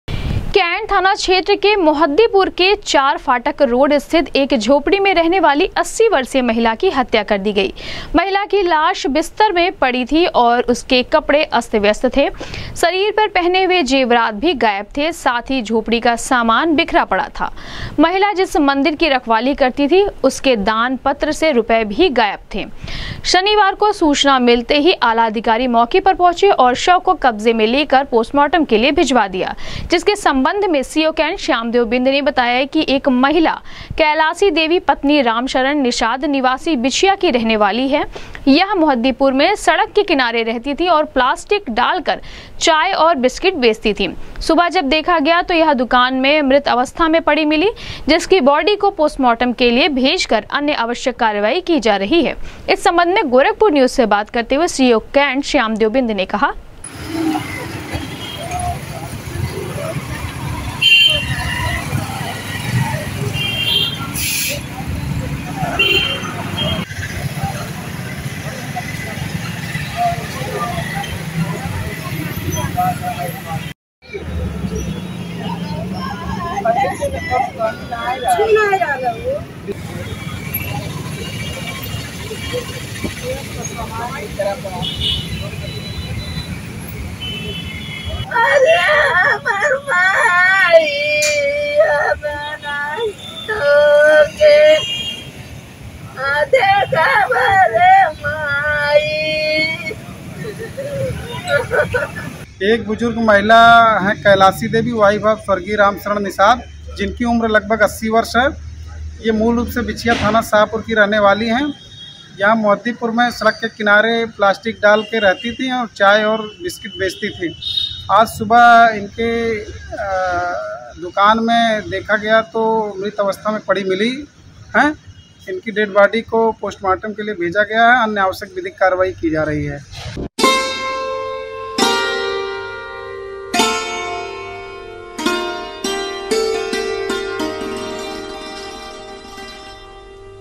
कैंट थाना क्षेत्र के मोहद्दीपुर के चार फाटक रोड स्थित एक झोपड़ी में रहने वाली महिला का मिला शव,घटना
कैंट थाना क्षेत्र के मोहद्दीपुर के चार फाटक रोड स्थित एक झोपड़ी में रहने वाली महिला का मिला शव,घटना की सूचना मिलने पर मौके पर पहॅुचे आलाधिकारी,सीओ कैंट ने कहा, शव को कब्जे में लेकर भेजा गया है पोस्टमार्टम के लिए,की जा रही आगे की विधिक कार्यवाही